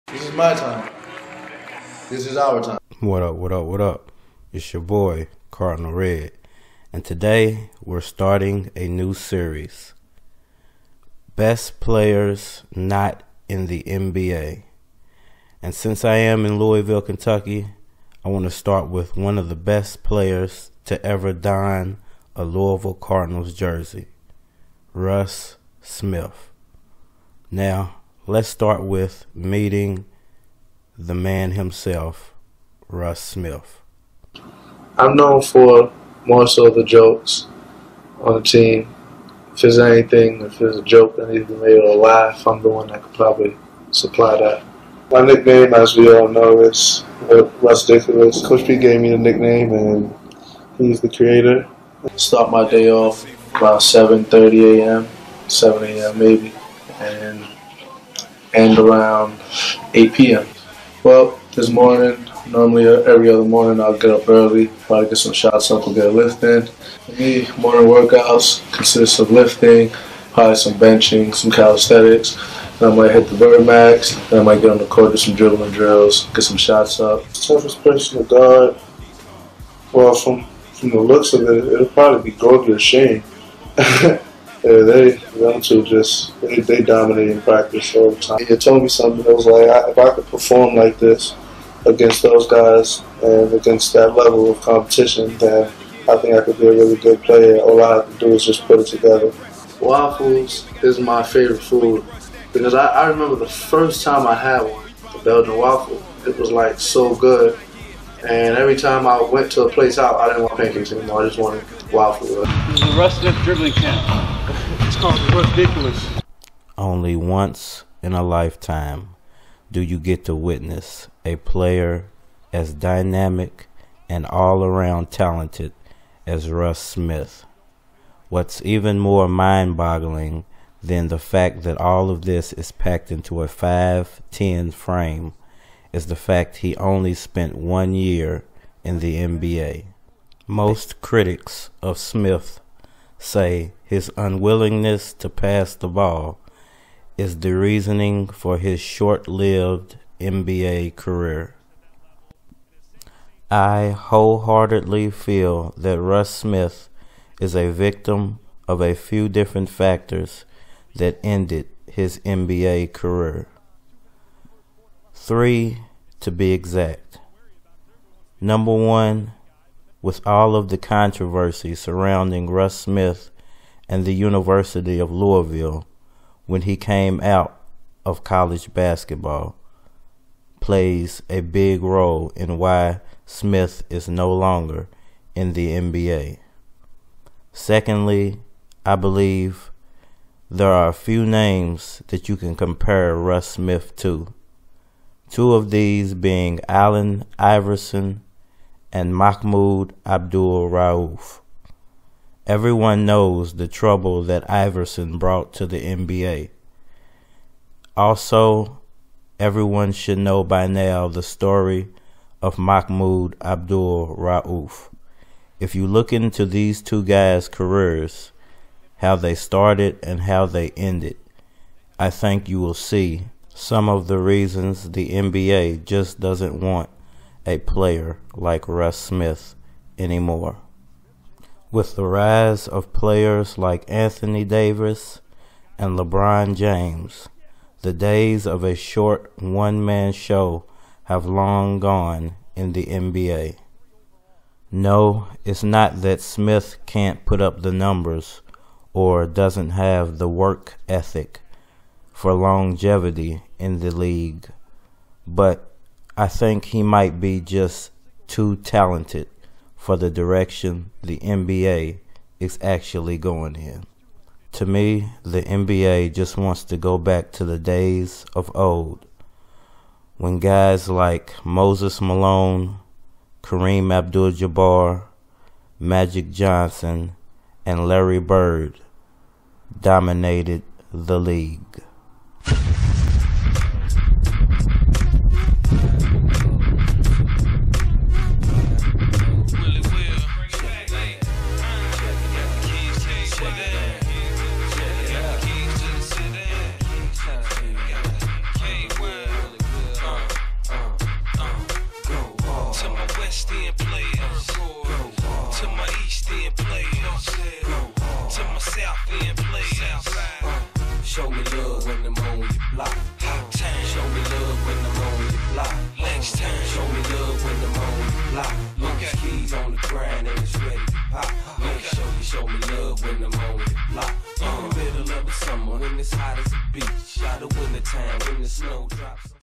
Best Players Not In The NBA | Russ Smith
Guard Russ Smith set the NBA D-League record with 65 points.
Russ Smith also won the 2013 National Championship with the Louisville Cardinals Basketball Team. The former Louisville star Shooting Guard spent less then one year with the Memphis Grizzlies NBA Basketball Team before gutting cut. He now plays in the NBL for the Guizhou White Tigers in China where he averages nearly 50 points per game. Best Players Not In The NBA will breakdown why Russ Smith is no longer in the NBA and why Russ Smith is the Best Player Not In The NBA.

Russ Smith Is arguably the best play to suit up for the Louisville Cardinals Basketball team. With the 2013 national championship on his record he solidified himself as as one of College Basketballs Greatest Shooting Guards.

Louisville Cardinal Weekly Report

By: Bryan Kalbrosky :Hoops Hype

Russ Smith averaged 61.2 points per game in the Chinese National Basketball League last season. Prior to Smith’s dominant campaign, no NBL player had averaged over 45 ppg.

Smith, the No. 47 overall pick in the 2014 NBA Draft and a former NCAA national champion, has played for the Memphis Grizzlies and New Orleans Pelicans. After playing out of his mind in the NBL, Smith has joined the Fujian Sturgeons of the Chinese Basketball Association for this upcoming season.

Earlier this summer, though, Smith said he “for damn sure” belongs in the NBA. Smith continued to display this confidence when he recently spoke with Dennis Chambers about his thoughts on not currently being signed to a roster in the United States.

I really think it’s BS that I’m not on an NBA roster, to say the least. There’s no way that I can look 30 teams in the NBA, in the states, and I can go, ‘All 13 guys on this roster is better than me.’ I can’t believe that.